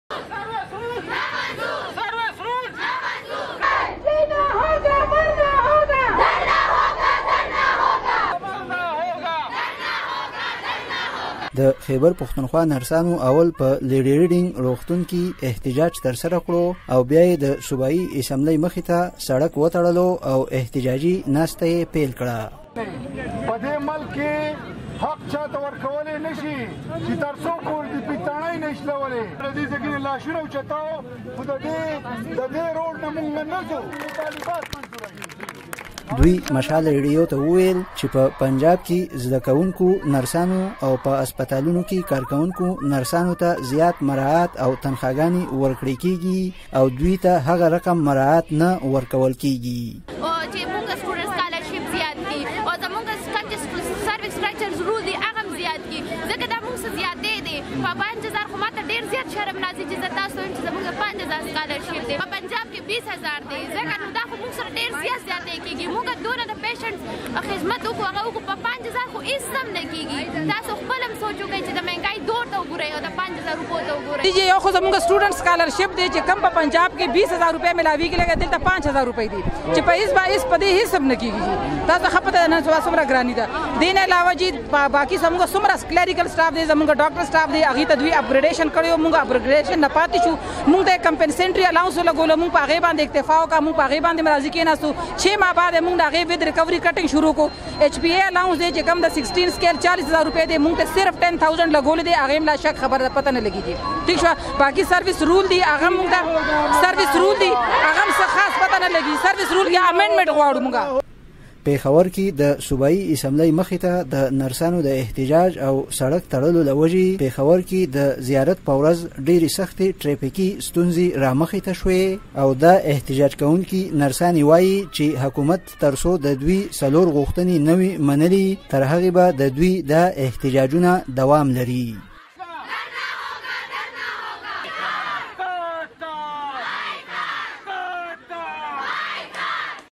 'د ناروغانو خدمت کوو خو تنخاګانې مو د نشت برابرې دي'
د خېبر پښتونخوا نرسانو صوبايي تنظيم د خپلو غوښتنو په حق کې د احتجاج په توګه له ننه د صوبې په ټولو هسپتالونو کې کاربندیز اعلان کړی دی. له پېښوره د مشال راډیو خبریال وايي چې نرسانو تېره ورځ (زیارت) د صوبايي اسمبلۍ مخې ته د خپلو تنخاګانو اضافې لپاره احتجاج وکړ او لوی سړک يې تر دولسو ګنټو د ګاډو پرمخ تړ